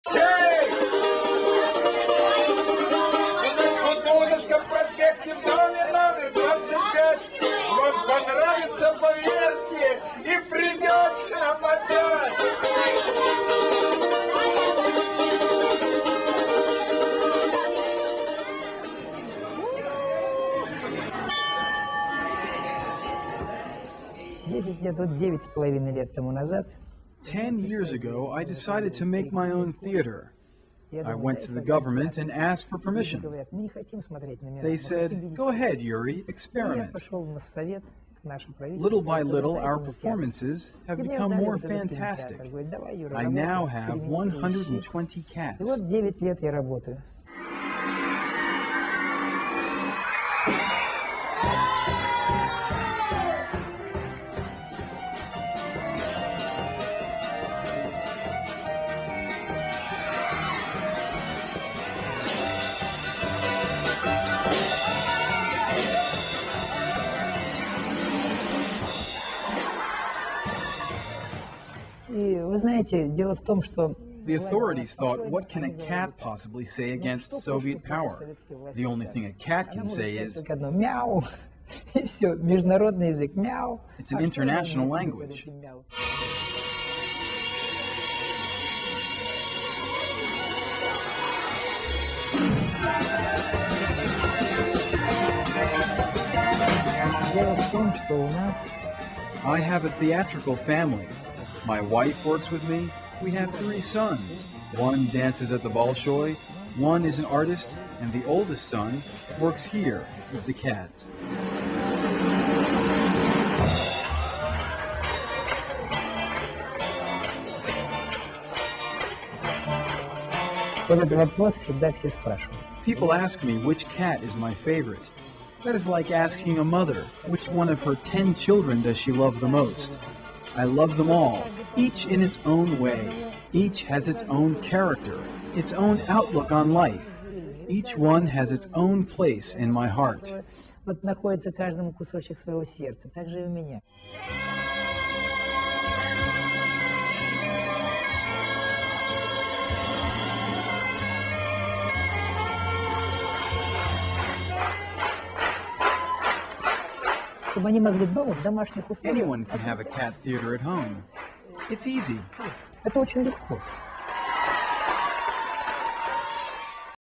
Moscow Cats Theater
Cats perform Cats at the Moscow Cats Theater.